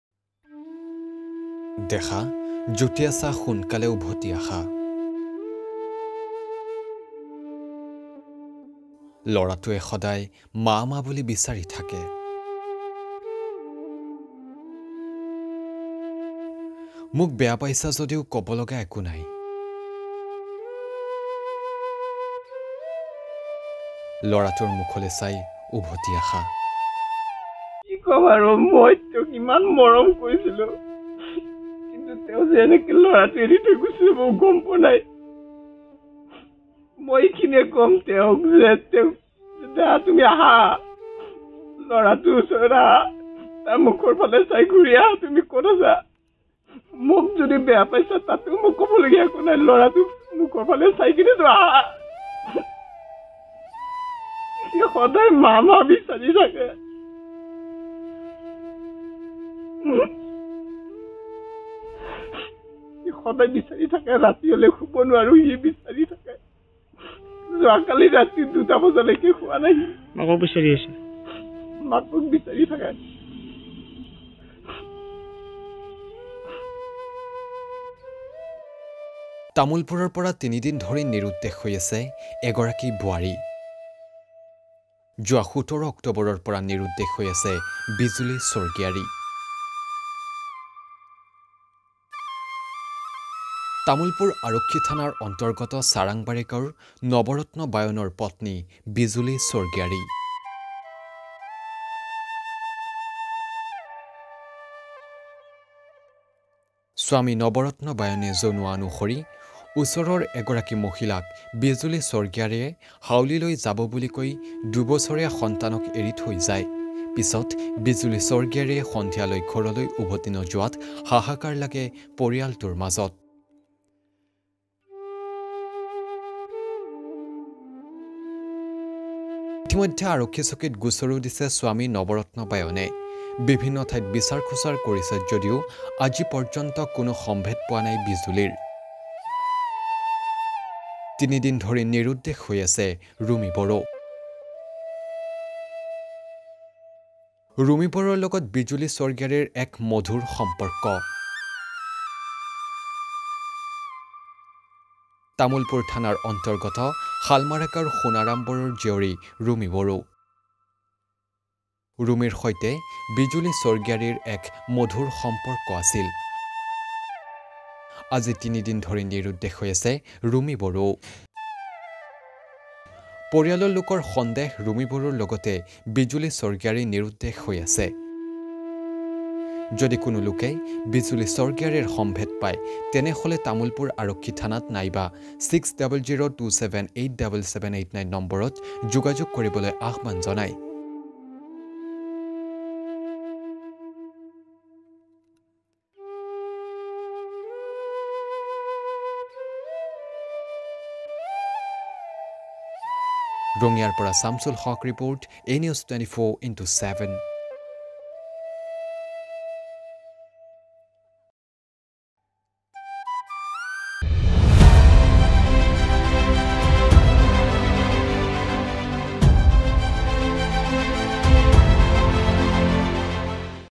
পত্নীক বিচাৰি স্বামীৰ কান্দোন
কণমানিটিৰ মুখলৈ চাই উভতি আহিবলৈ আহ্বান
তিনি দিন ধৰি নিৰুদ্দেশ হৈ আছে বিজুলী স্বৰ্গীয়াৰী